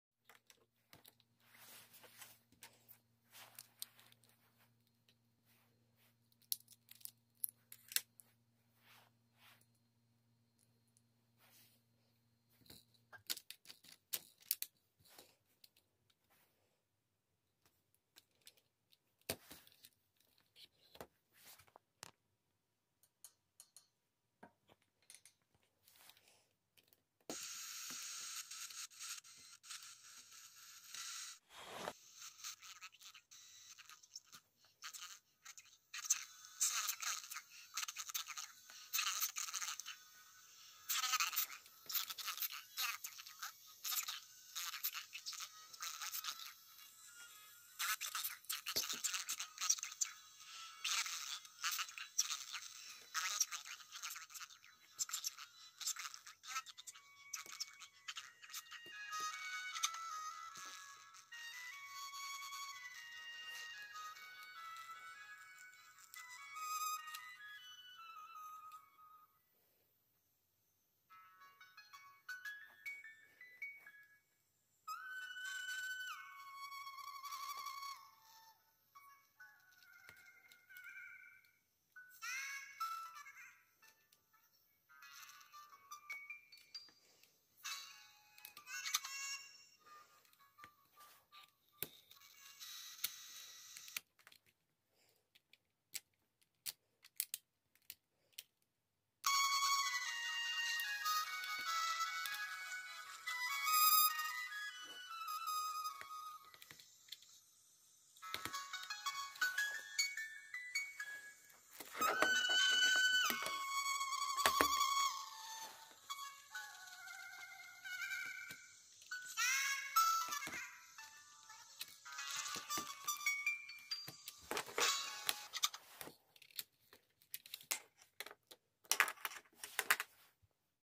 미니 붐박스 장난감_ 가지고 놀기 [라디오OK 녹음OK브루투스X 음량조절OK]
쇼와 레트로 미니 카세트 라디오 장난감 입니다. 붐박스라고도 하지요.
소리는 별로지만 가지고 놀기 신기하고 재미 있습니다.